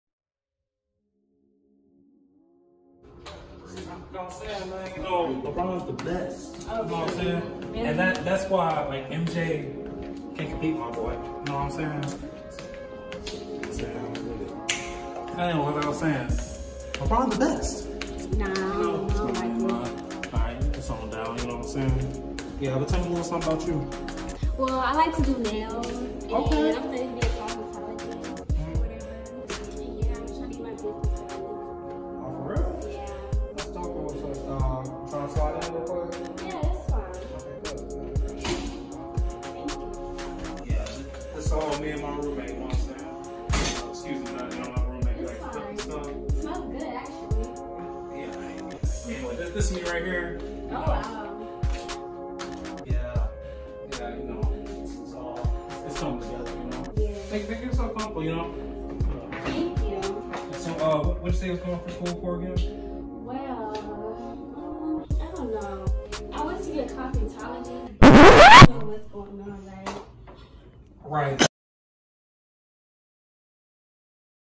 Almost Perfect Date
What happens when Ajs date doesn’t go as planned lol comedy newvideo new #2020